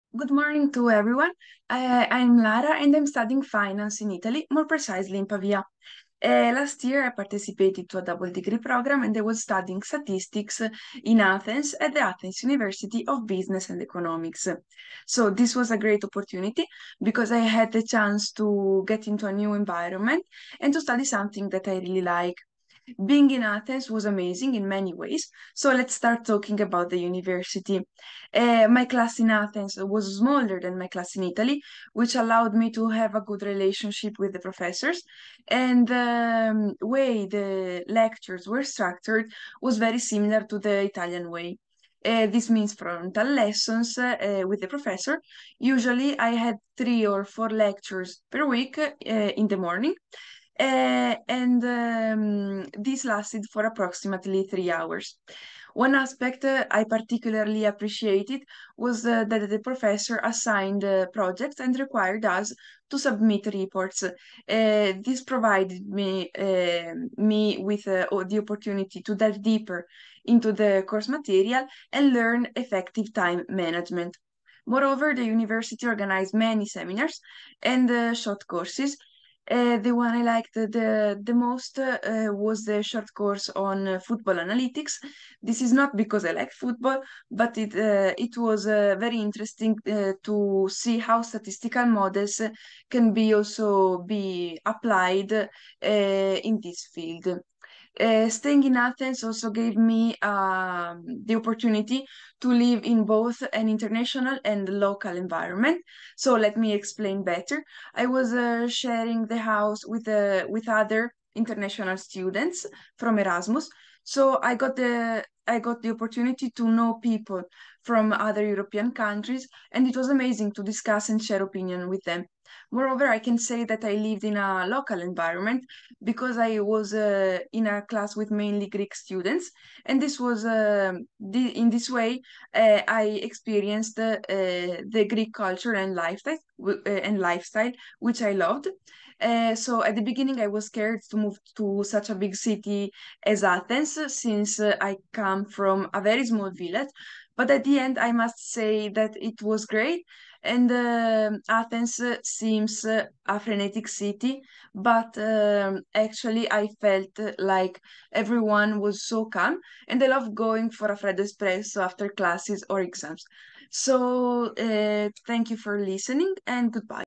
AUEB’s very first Double Degree Student!
🎉 Lara Nichetti is AUEB’s very first Double Degree Student! Lara is now finishing her Double M.Sc. Degree in Statistics & Finance, comprising two Degrees: a Master of Science in Statistics, from AUEB and a Master of Science in Finance from the University of Pavia.

💡Lara reported that she was quite excited and satisfied by her overall educational experience at AUEB: “One aspect I particularly appreciated was that the Professors assigned Projects and required us to submit reports. This provided me with the opportunity to dive deeper into the course material and learn effective time management. Moreover, the University organized many Seminars and Short Courses – the one I liked the most was the Short Course on Football Analytics; this is not because I like football, but, it was very interesting to see how Statistical Models can also be applied in this field”, Lara states among others.

🔵 ⚪ However, it wasn’t just the University that Lara seems to have thoroughly enjoyed! “At the beginning I was scared to move to such a big city as Athens, since I come from a very small village, but, at the end, I must say that it was great … I felt like everyone was so calm and I love going for a freddo espresso after classes”.

Professor Ntzoufras, Head of the Department of Statistics at AUEB, states that he is delighted that our dream comes true. “AUEB has now new international modern integrated postgraduate programs. This is the results of hard work, collaborations and negotiations of many years. Lara, our first incoming student from the University of Pavia, was exceptional both in terms of academic perfomance and terms of personality. Apart of the academic part, I really liked that Lara interacted with the rest of the students and created friendships for a lifetime. This academic year, two of our students (the first outcoming AUEB DD students) will follow courses in the University of Pavia and I am looking forward to knowing details about their experience. This is the beginning of a new era where more international students will visit AUEB and the Department of Statistics for studying to our excellent postgraduate programs.”

🌍 Double Degrees constitute educational opportunities for state-of-the-art Double Degree Studies and cultural exchange and we could not have been more proud of such initiatives!